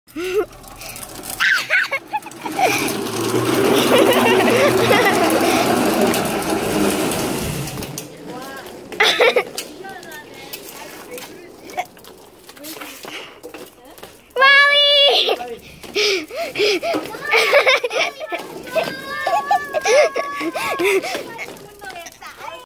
Our Dog Loves Shoelaces
Molly loves to chase shoelaces!